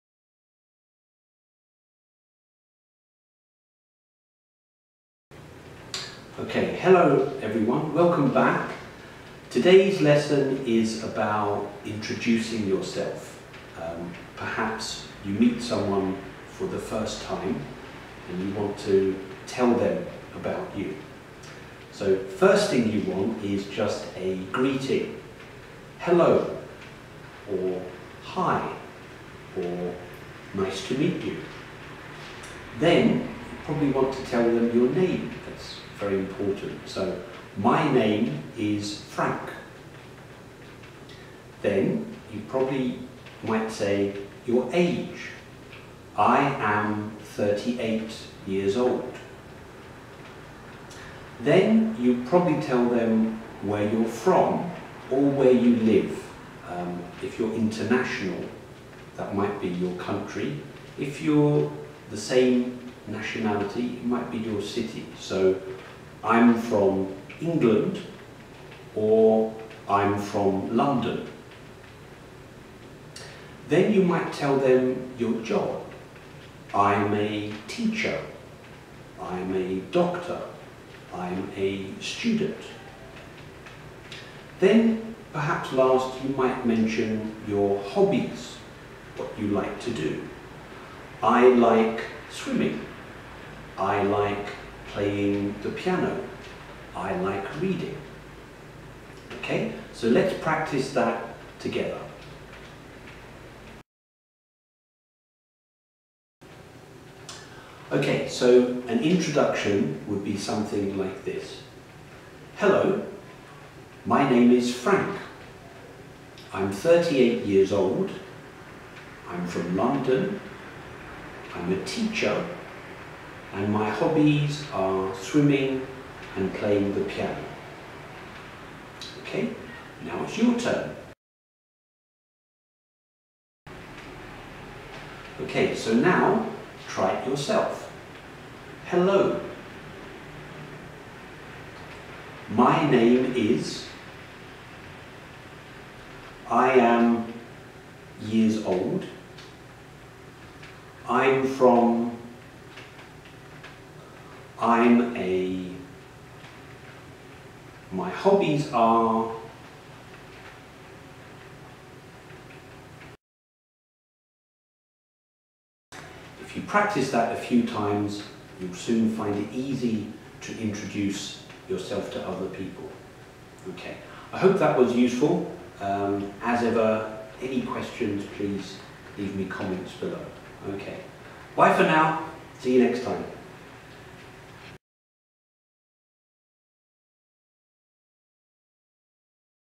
English for beginners: introducing yourself
This video will help you introduce yourself to new people.